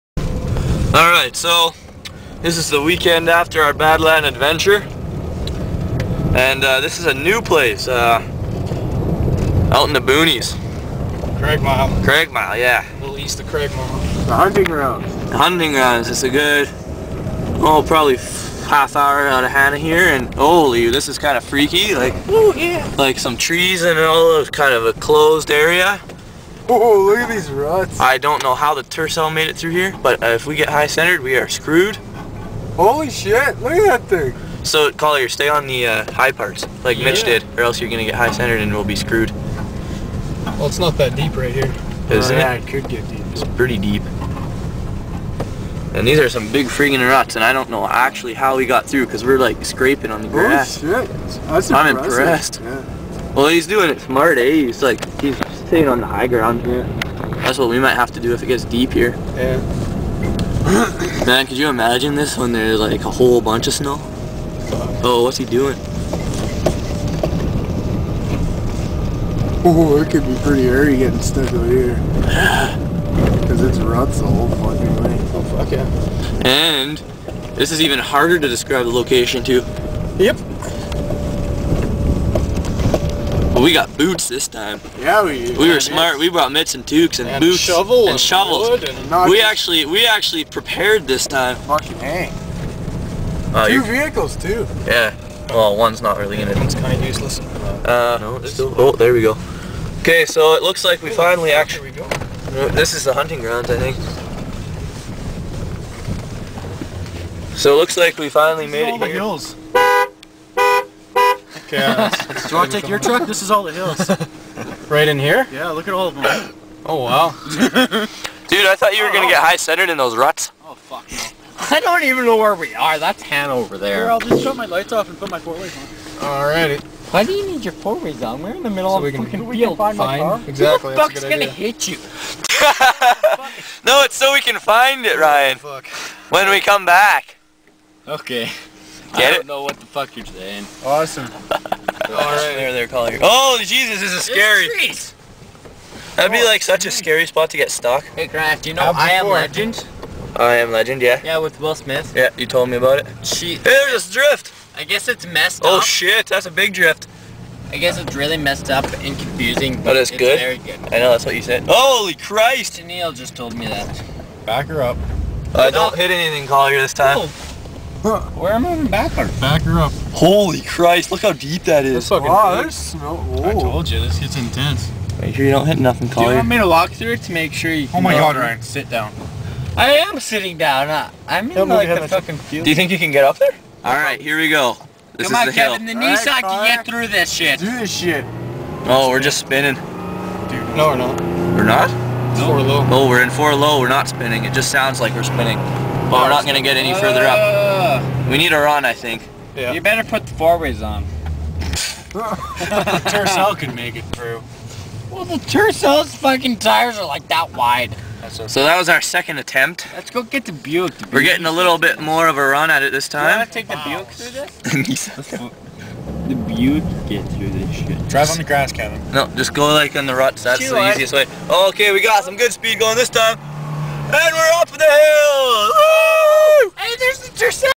Cragmyle Nissan Frontier Off-Road Compilation
Another night of fun east of Cragmyle with some friends, my truck, and my friend's Tercel.